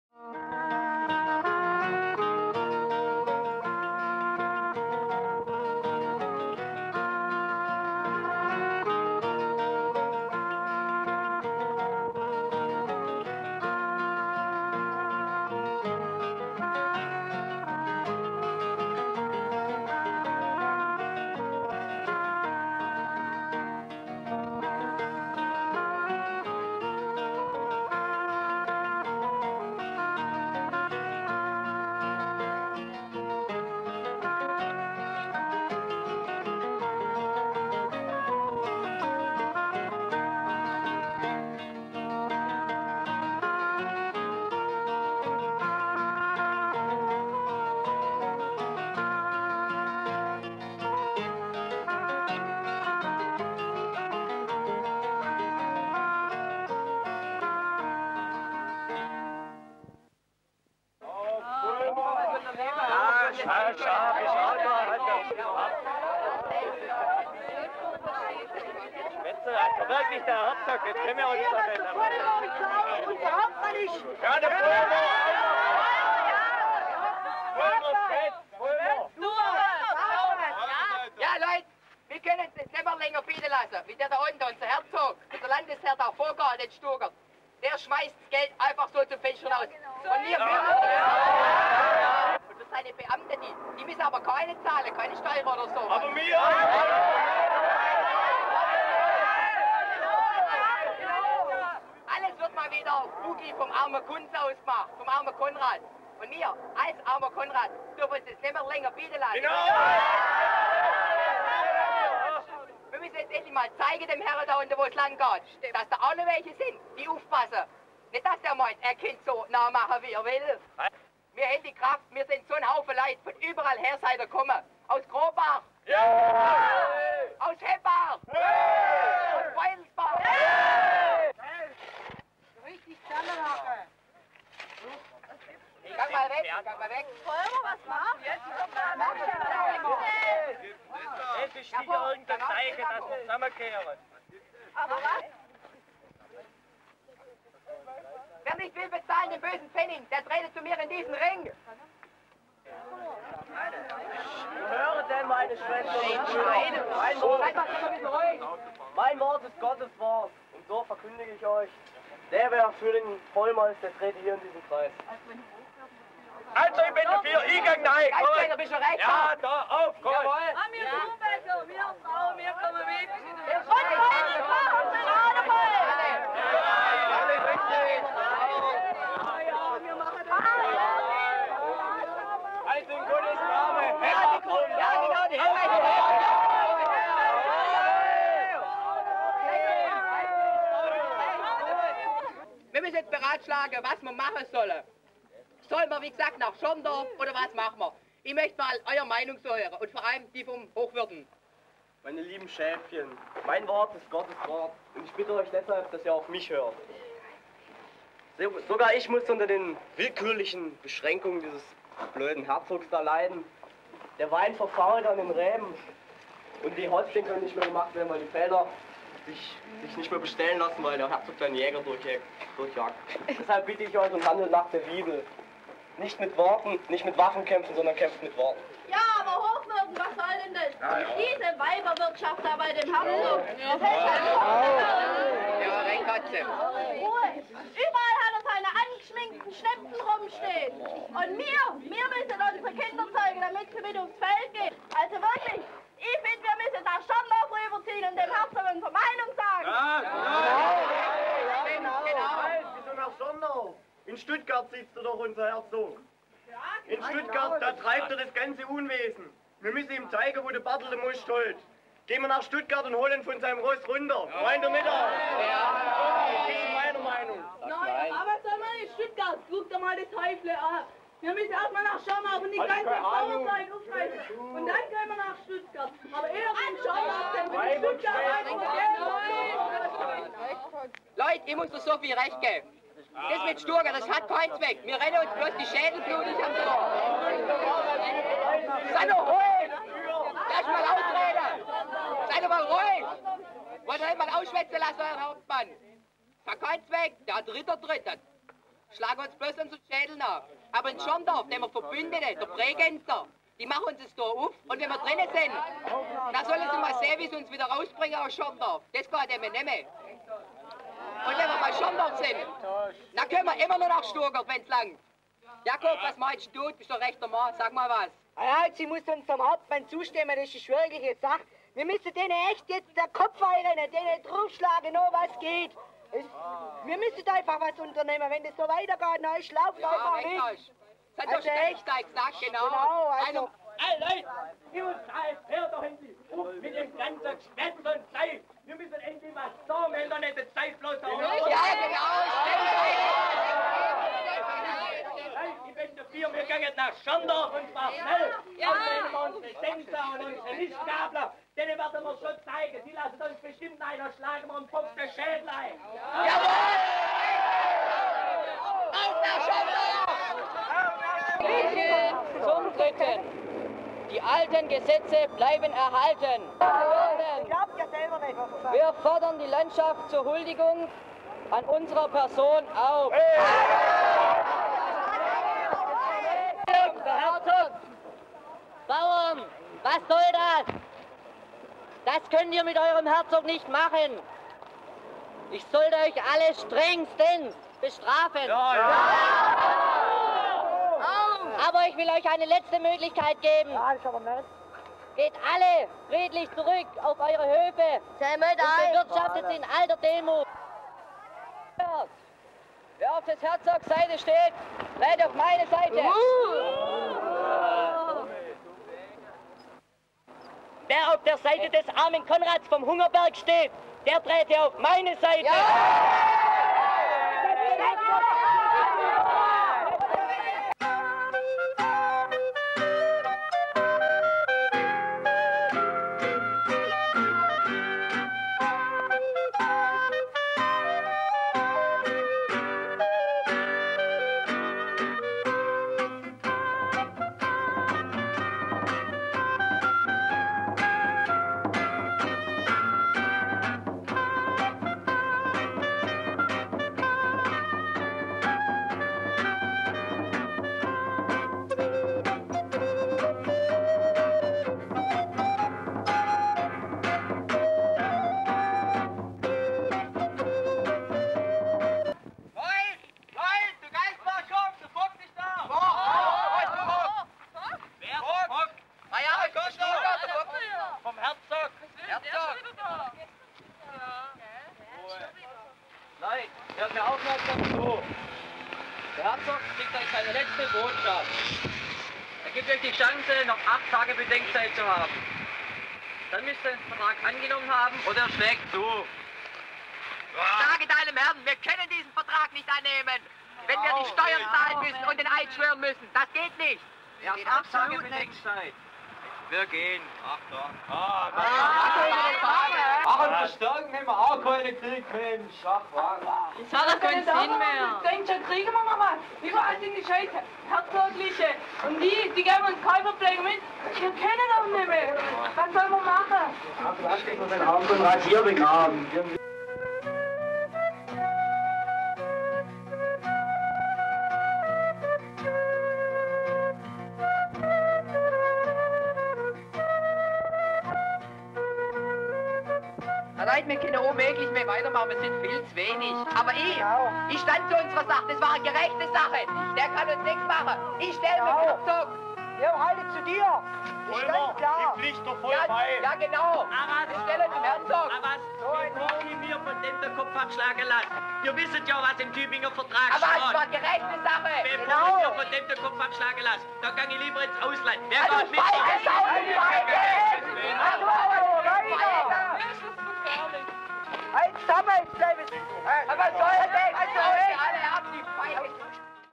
Der Arme Konrad
Ein Unterrichtsprojekt in Geschichte aus dem Jahre
1980 produziert mit Super 8 Film